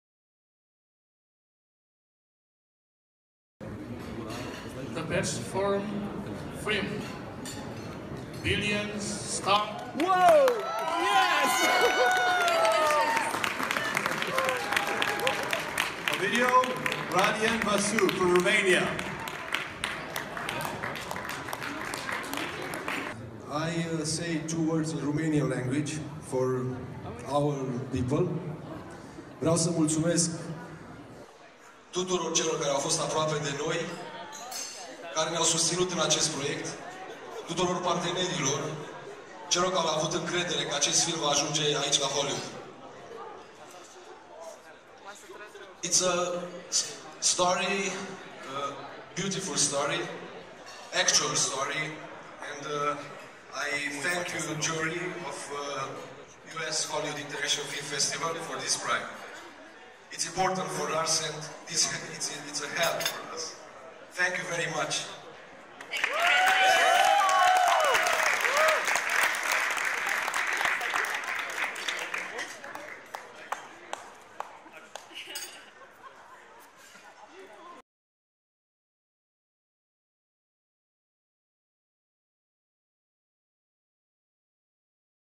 Billion Star Hotel - Cel mai bun film strain la US Hollywood International Film Festival
Discurs Ovidiu Vasu, la decernarea premiului pentru cel mai bun film strain la festivalul US Hollywood International Film Festival.

Regia: Alecs Nastoiu
Producator: Ovidiu Vasu
O productie GAT Films Transilvania

Sinopsis film:
În fiecare zi, ne este dat următorul exercițiu: să dăm lumii din jurul nostru o turnură. Să zâmbim, să luam totul în glumă, să rădem cu poftă. Sau să ne lăsăm copleșiți de realitate, să fim conștienți de distanța dintre noi și visele noastre, de piedici și de faptul că suntem singuri în tot acest timp. Unii aleg să evadeze, să-și construiască propria lume cu propriile reguli, cu amintiri din basme și dorințe care se împlinesc. Sau poate totul se rezumă la a visa cu ochii deschiși și a umple spațiile goale cu muzică. Totul ține de tine și de felul în care alegi să privești lucrurile: în alb, negru sau în pași de dans. Billion Star Hotel - un film cu atâtea povești cât spectatori.